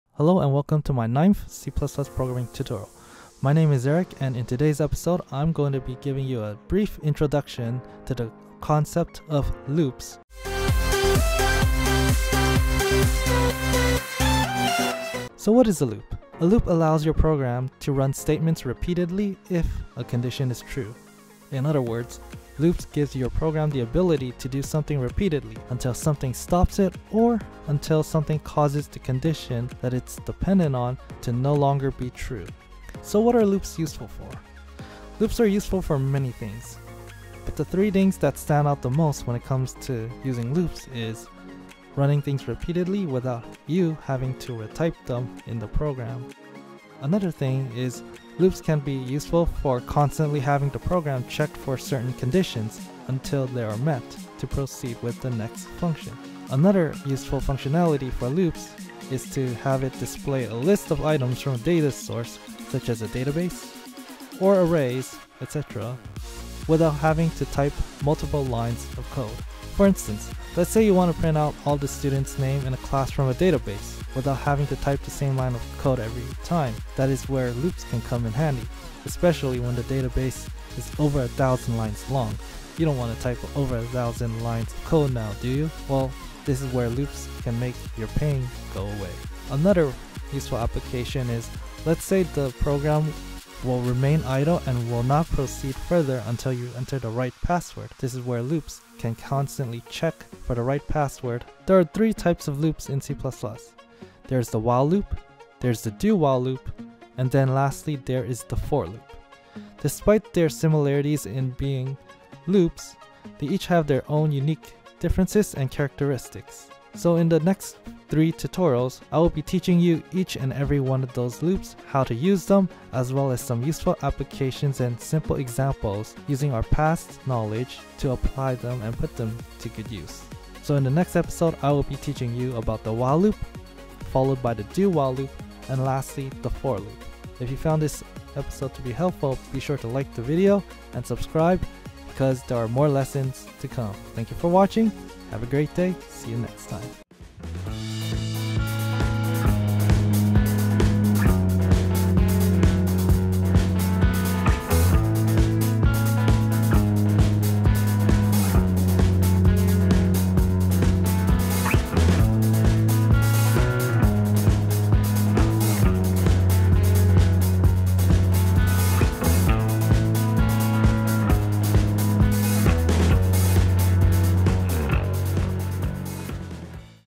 C++ Programming Tutorials - 9 - Intro to Loops - Eric Liang
In this C++ Programming tutorial for beginners, I will be teaching you a general overview of loops in C++. Loops allows you to run certain pieces of code repeatedly until something either stops it or until the conditional statement it's dependent on is no longer true. Then in the following 3 video lessons, you will learn about the three types of loops in depth and put them into good use. The 3 loops in C++ are while loops, do while loops, and lastly the for loop.

The introduction music used in this video is created by Tobu and Itro.
I do not take any credits for the creation of the music content.